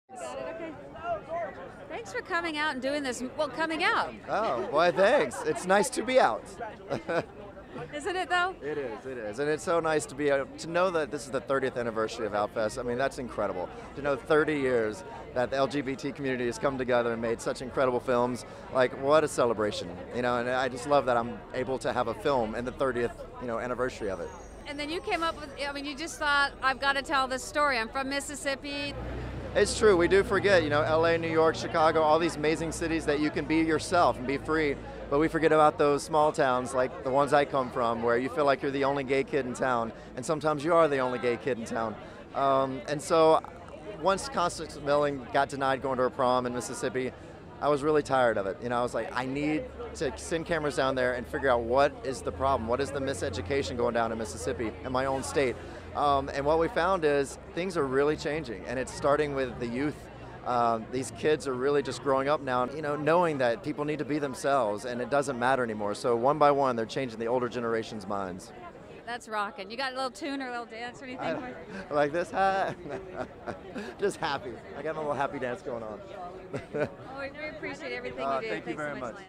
Lance Bass ♡ MISSISSIPPI: I AM documentary Outfest
Lance Bass, inspired by a lesbian teenager who sued her Mississippi high school for the right to take her girlfriend to the prom, produced a powerful new documentary MISSISSIPPI: I AM.  Bass, a native of Laurel, Mississippi, says "... L.A., New York, Chicago all theses amazing cities that you can be yourself and be free but we forget about those small towns like the ones I come from where you feel like the only gay kid in town and sometimes you are the only gay kid in town!" Lance will be in attendance at the film screening tomorrow in L.A. Outfest Film Festival, Saturday, July 14, 11:30am at the DGA 2 / Video by Renee Sotile & Mary Jo Godges of Traipsing Thru Films